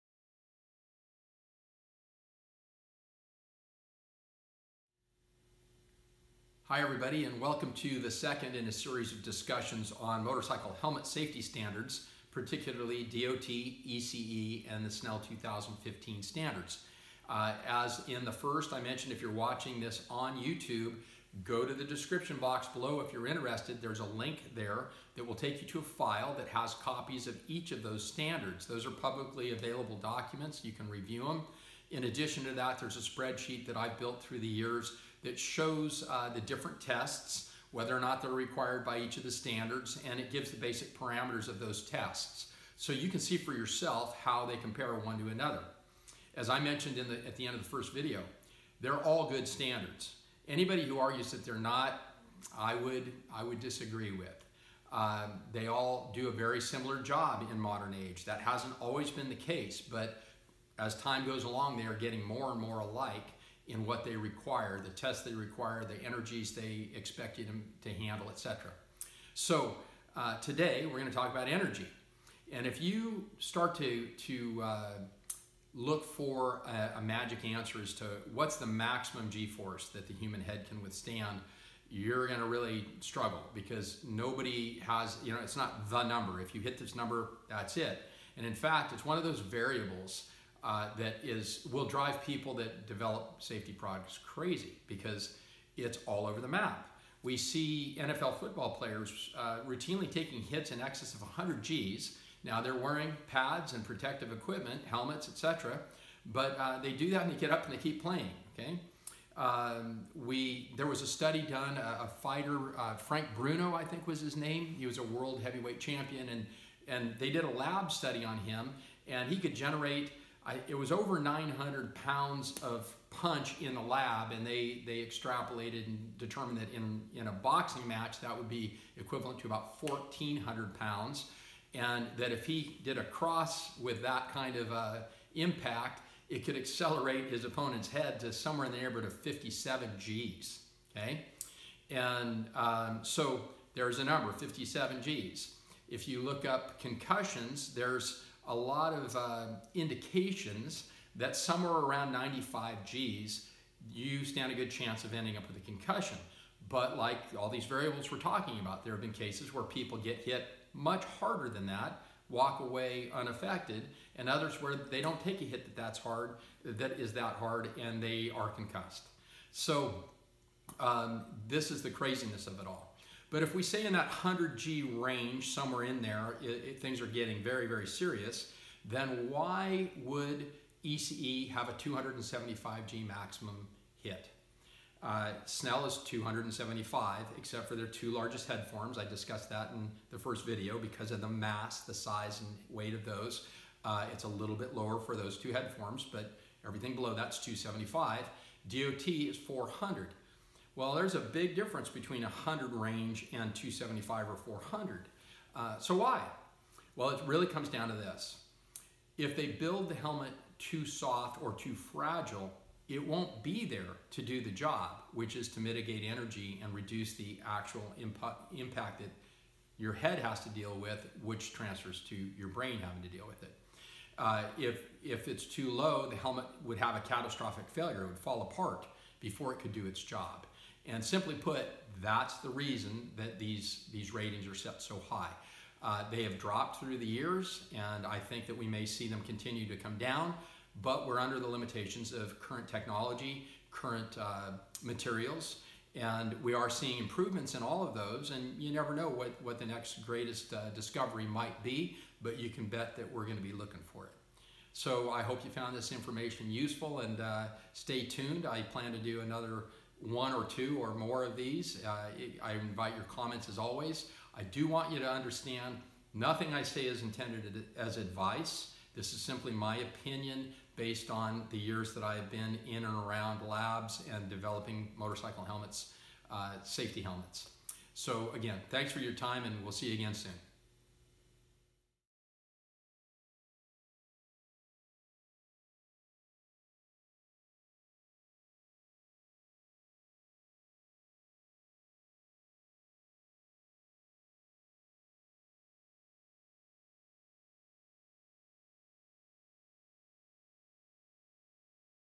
PART 2 DOT vs ECE vs Snell Motorcycle Helmet Safety Standards Ratings. Why 275 to 400 G's???
DOT vs Snell vs ECE - What are the differences and similarities between the DOT FMVSS 218, ECE 22.05 and Snell 2015 motorcycle helmet safety standards? This video series does its best to explain the standards, and clear up some misconceptions. FREE STANDARDS DOWNLOAD. See the LINK at the bottom of this description.
In this video, the second in the series, the speaker explains why motorcycle safety helmet standards allow such high peak impact forces, relative to what the human form can withstand. These are only the opinion of the speaker and not intended as advice.